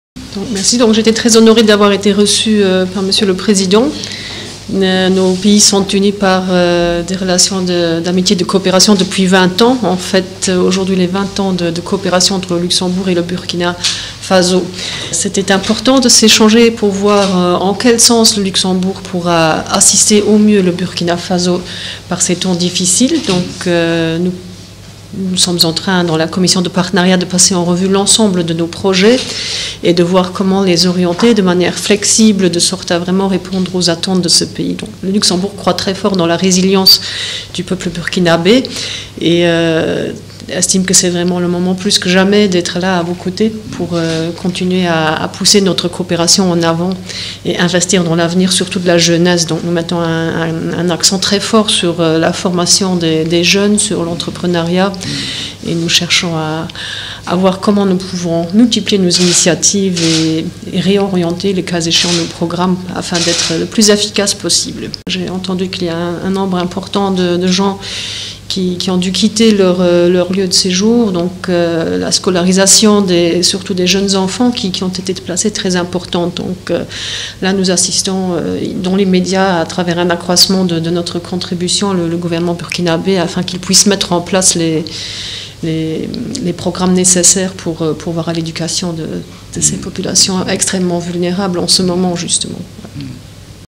Ministre de la cooperation au développement du Luxembourg
(Ouagadougou, 21 octobre 2019). Le président du Faso, Roch Marc Christian Kaboré, a accordé en fin de matinée, une audience à la ministre de la Coopération au développement et de l’Action humanitaire du Grand Duché de Luxembourg, Paulette Lenert.